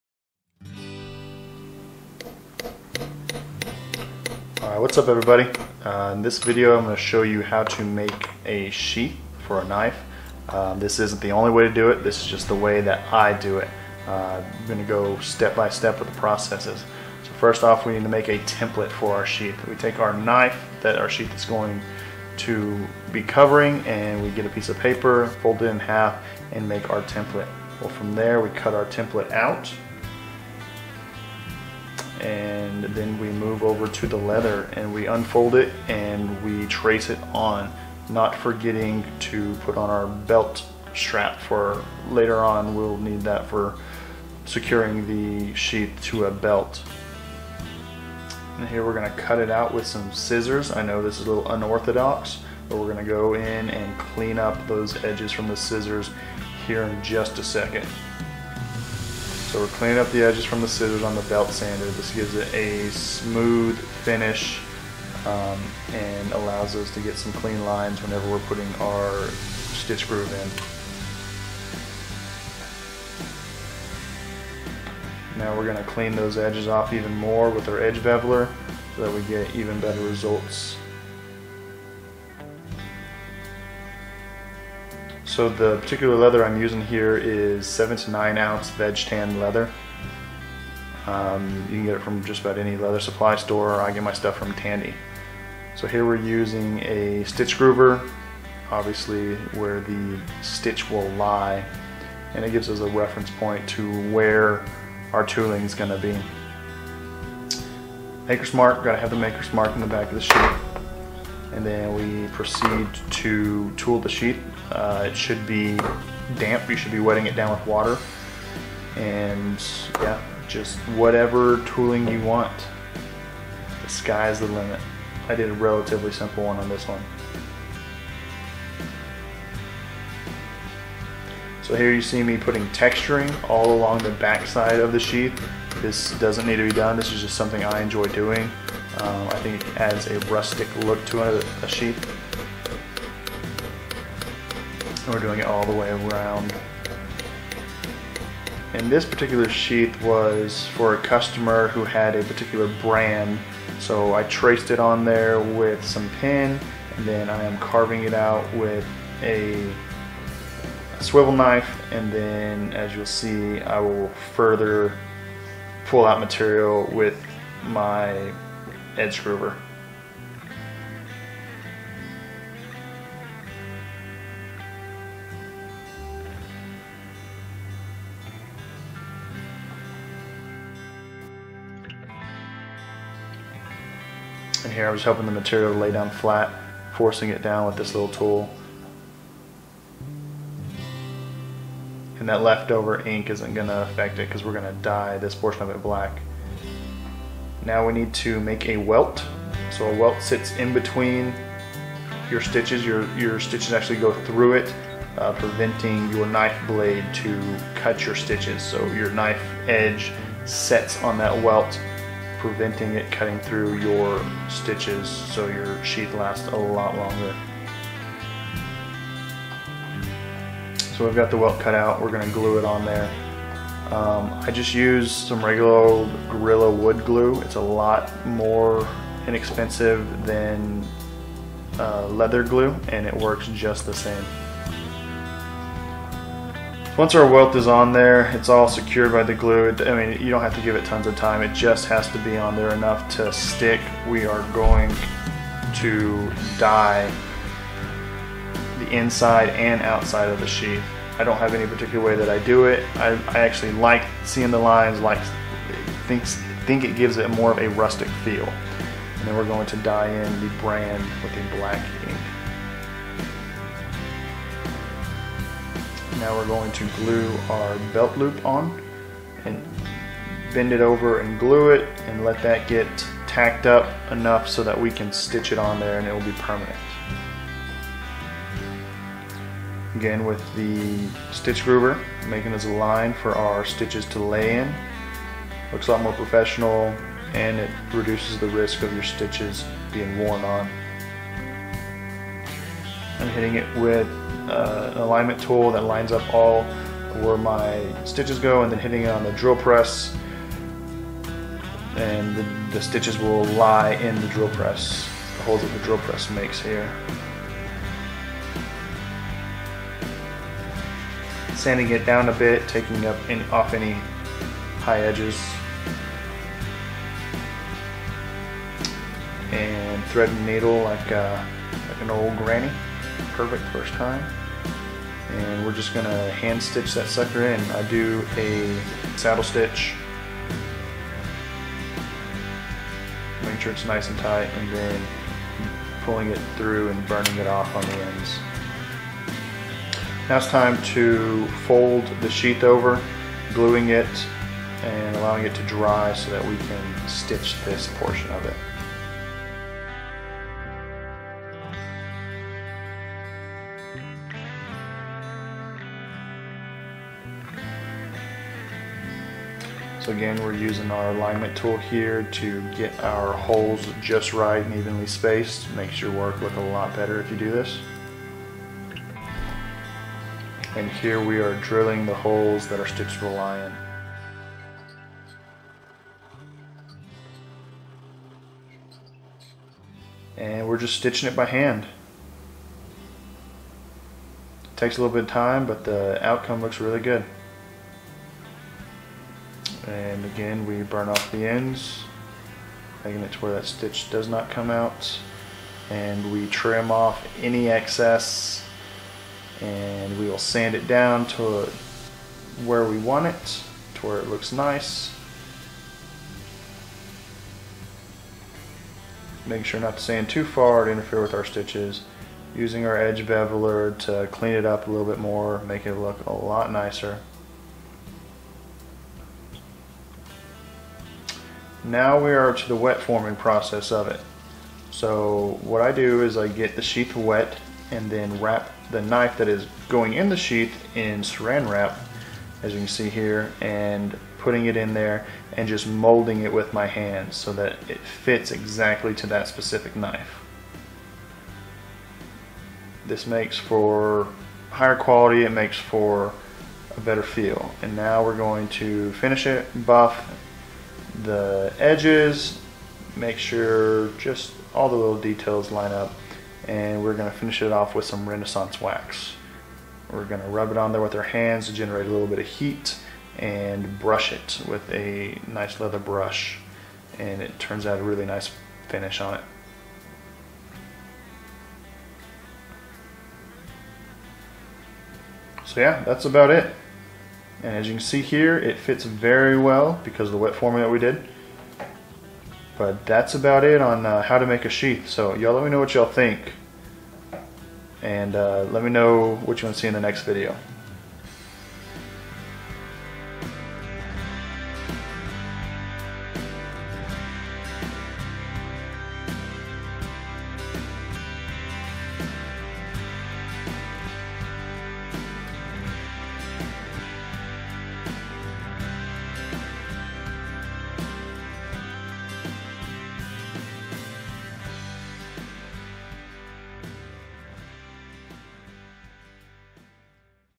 How to make a leather Knife Sheath - DIY Tutorial
A step by step DIY tutorial on how to make a leather knife sheath. Complete with footage and voiceover to give you a look at how I do all of my knife sheaths. I show my process of fitment, tooling, dying, and stitching!